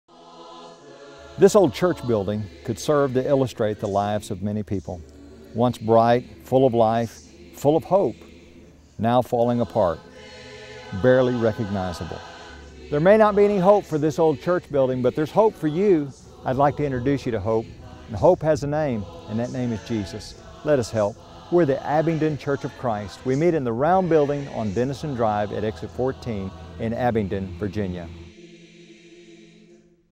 There is Hope For You
No matter what has happened in your life, there is hope. Hope has a name, and that name is Jesus.